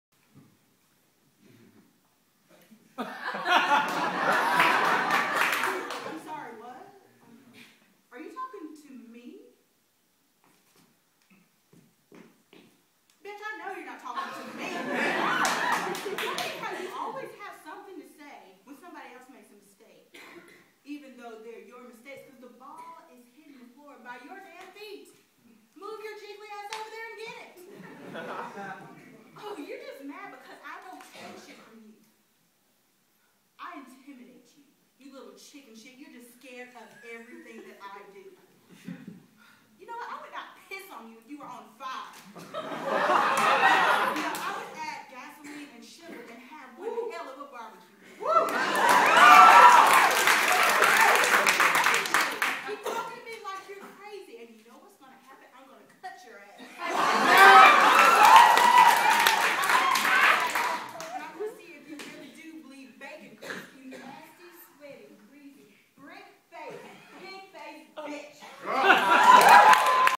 I Should Have Said - Phoebe Threat
Phoebe Threat performs an original monologue entitled, "I Should Have Said."

Guerrilla Theatre Dec. 4, 2010